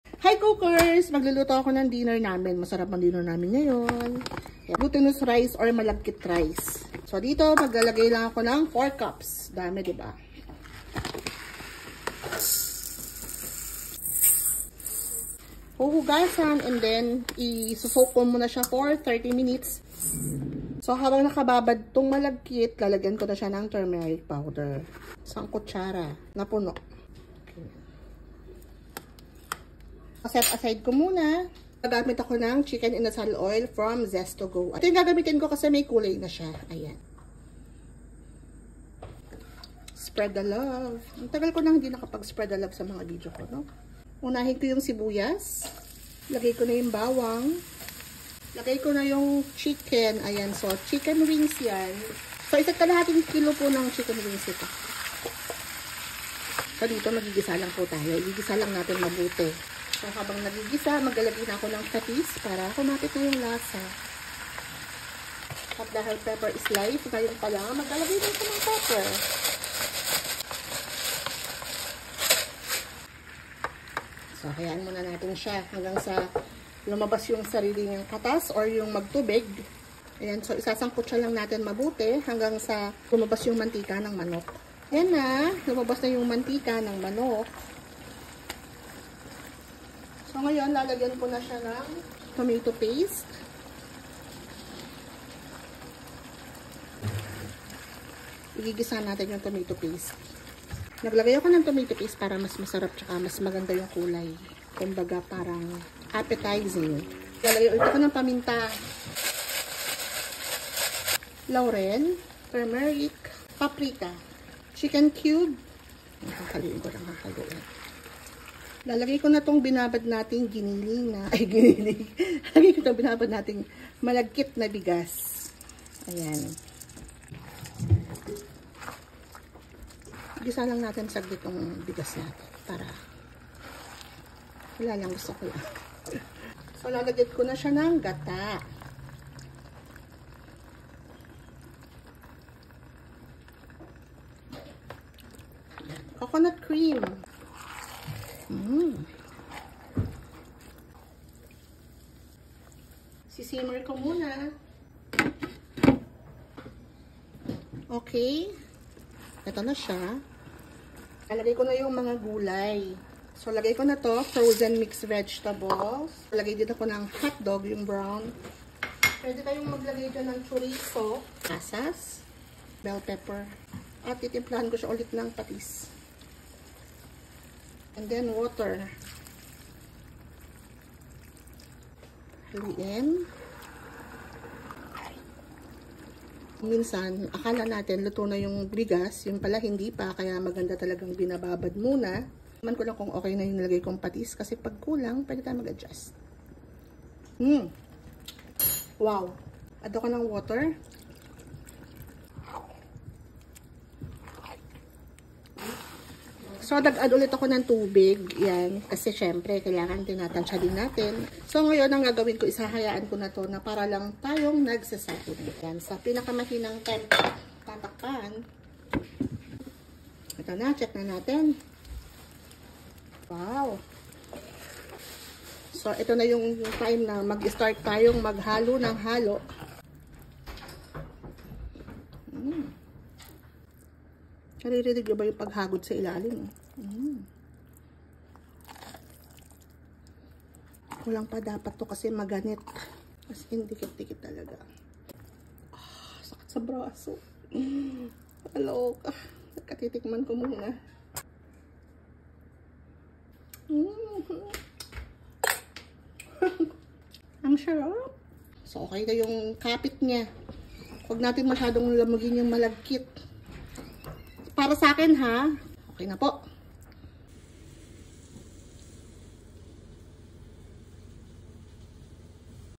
GAWIN MO TO SA MALAGKIT NA BIGAS SOBRANG SARAP!!!
Gawin mo to sa malagkit na bigas sobrang sarap. Pwedeng pwede pang handaan.

ARROS VALENCIANA
ARROS PAELLA

Arros Valenciana Recipe

4 cup’s glutinous rice- soak in water with turmeric powder in 30 mins

1 large onion
1 whole head minced garlic
2 tbsp tomato paste
1 tsp turmeric powder
1 tap paprika
3 pcs bay leaves
1 chicken cube
3 tbsp fish sauce or according to your taste
400g coconut cream
3 cups water- add if needed
1 cup frozen mixed vegetables
1 cup sliced hotdogs
1/2 cup raisins
1 bell pepper
• adjust the taste to your liking